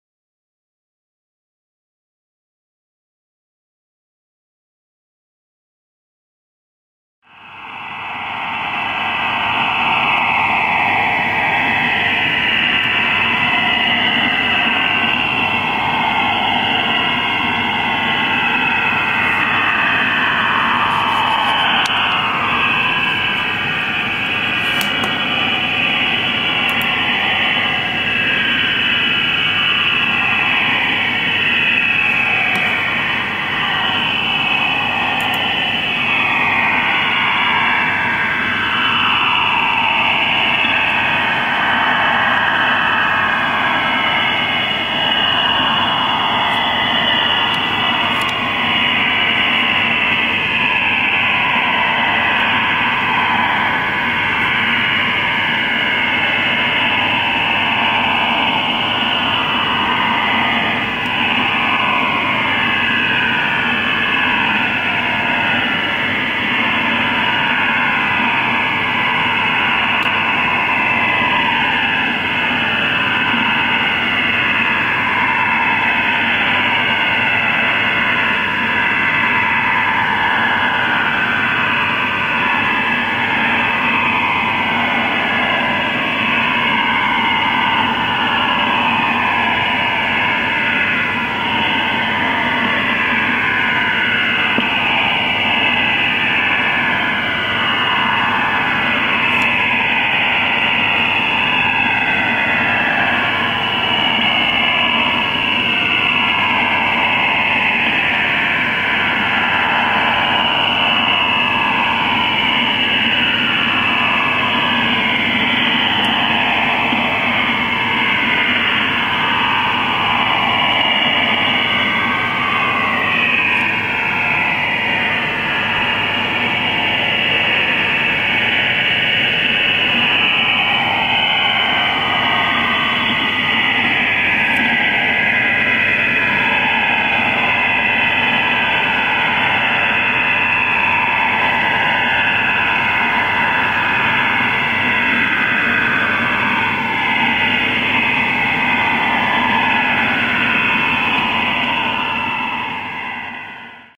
IDR Rome Stanag 4285 - Shortwave 6333Khz
Reception Location, Mansfield, Nottinghamshire, UK
Fri 11 July 2014
Stumbled upon while playing with STANAG decoder in MultiPSK software.